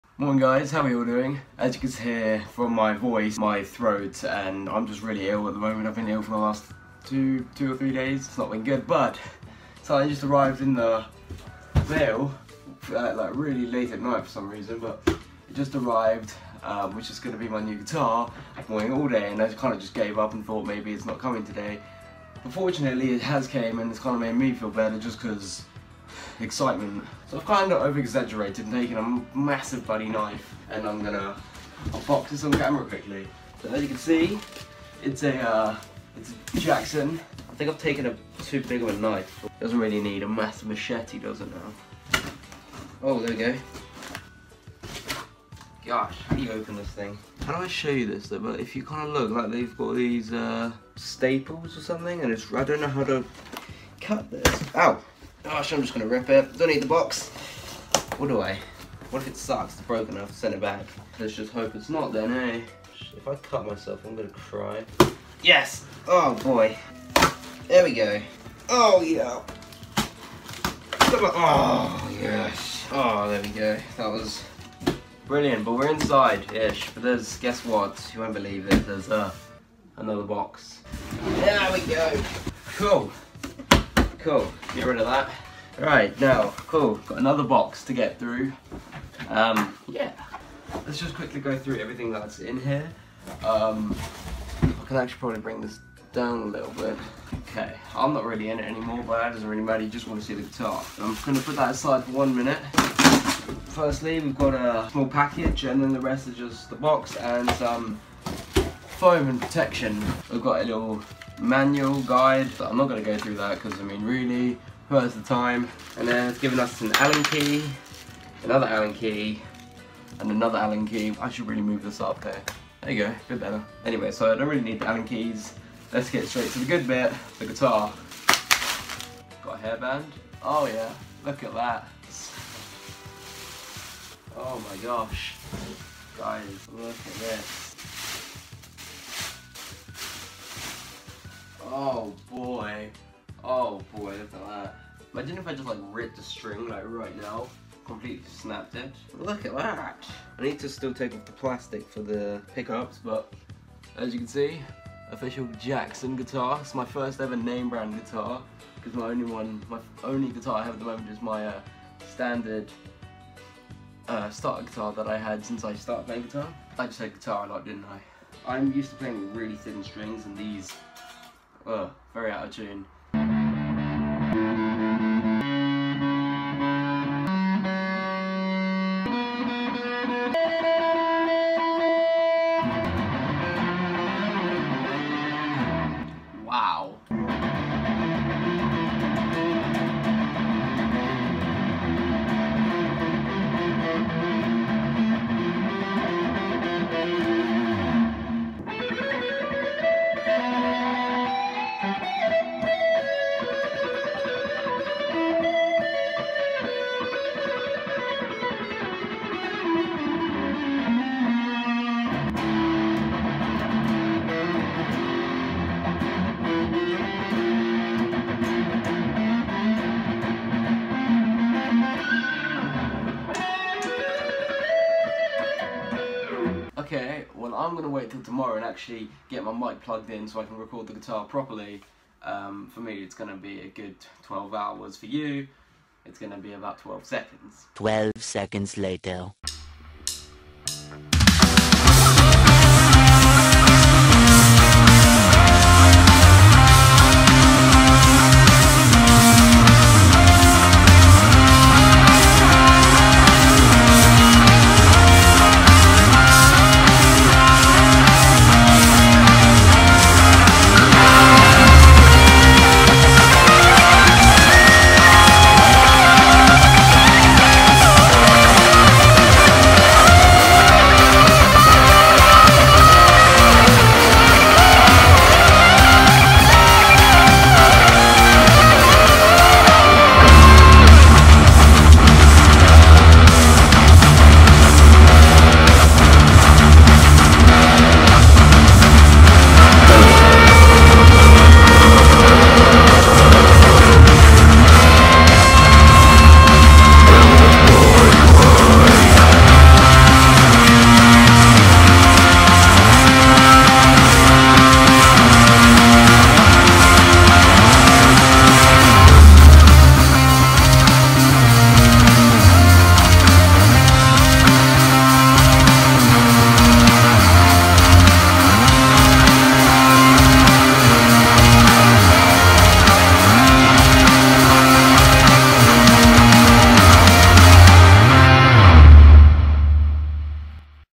My NEW Guitar *Unboxing and Review/First Play* (Jackson Dinky JS22 DKA)
*Skip to 3:59 for the review*

►  So after 6 years of playing i finally got my second electric guitar, the Jackson Dinky JS22 DKA, What are your thoughts? ▼